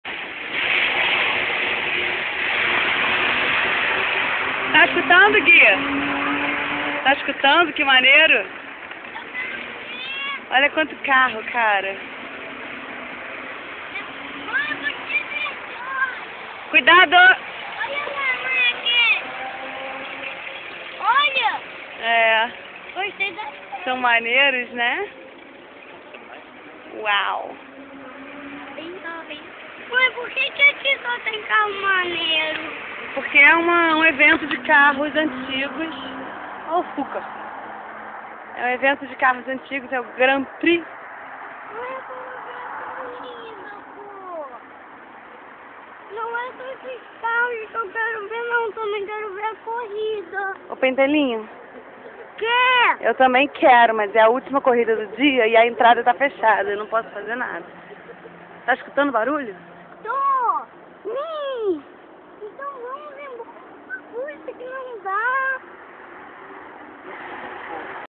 Copenhagen Historic Grand Prix
Não conseguimos entrar pra assistir a corrida mas demos uma espiadinha nos carros... WAUWWWWW! Muito legal!!! =)

Nao sei porque, mas o filme tá 'ao contrário', rsss. O celular é novo e eu nao sabia que se vc filmar CERTO, ele fica tombado. Ha ha. Que burra. =P

Enfim. Gui tb tava reclamando PACAS, mas só coloquei o filme aqui pra constar... Rs.